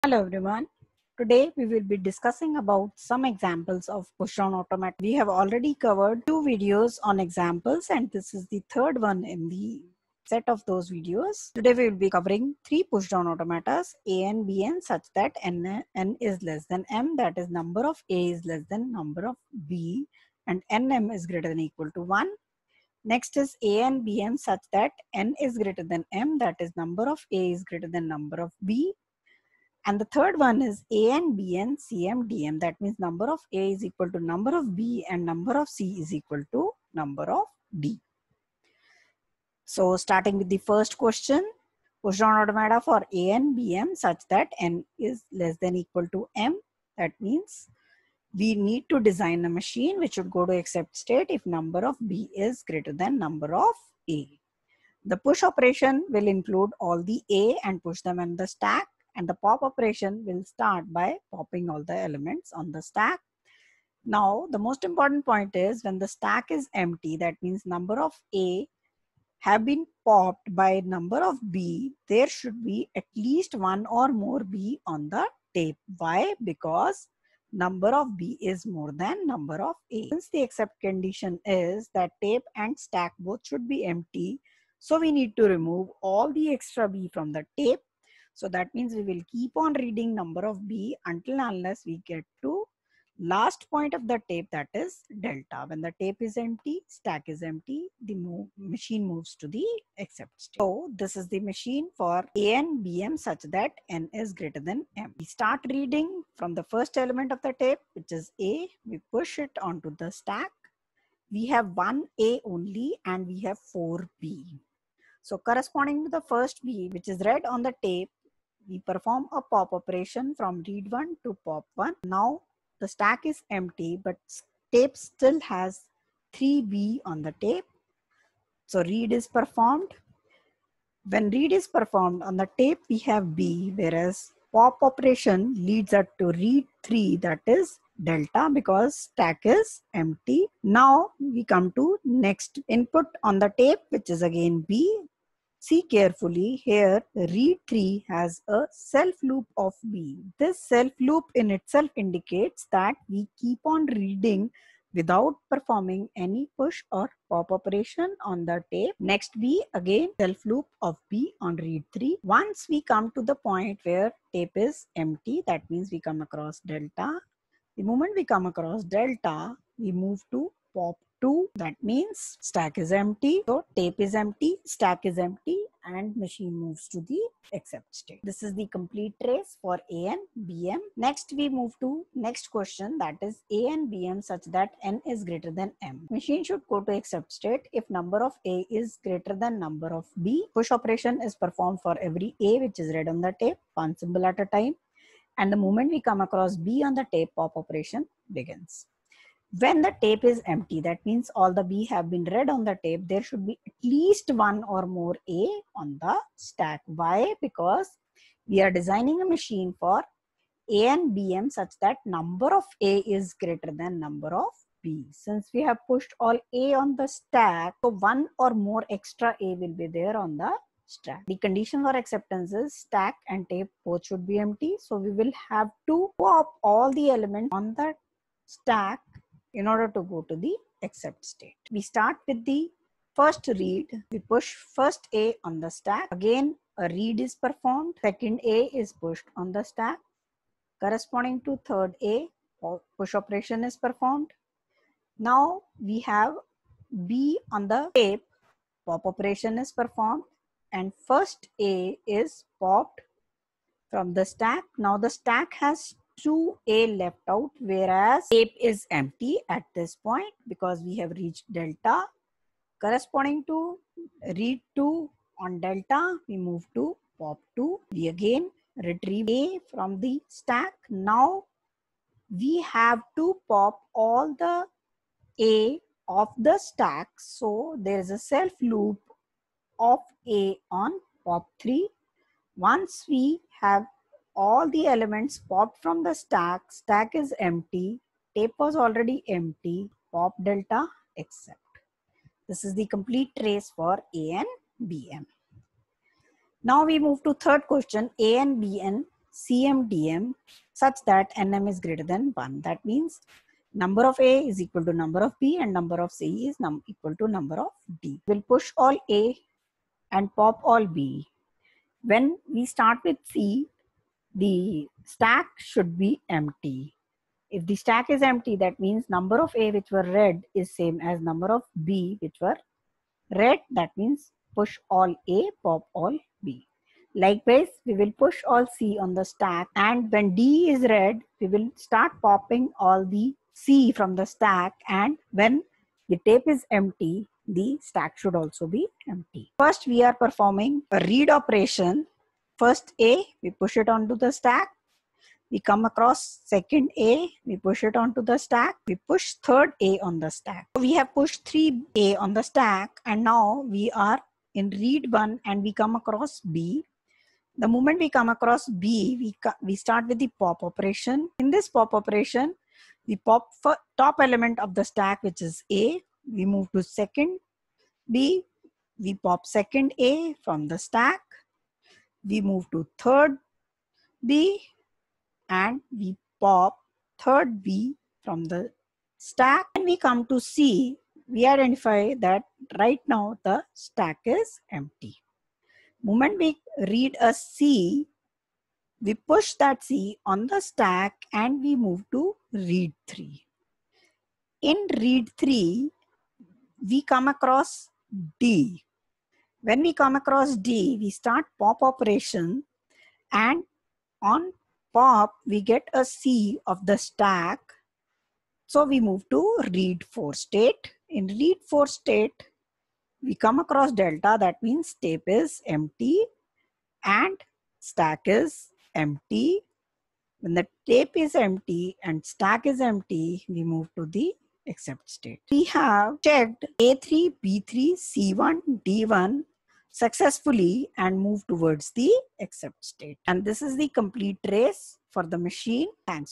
Push Down Automata Ex. 3 (Method 1)
Pushdown Automata Examples, a^nb^m|n is greater then m; a^nb^m|n is less then m; a^nb^nc^md^m, number of a and b are same and number of c and d are same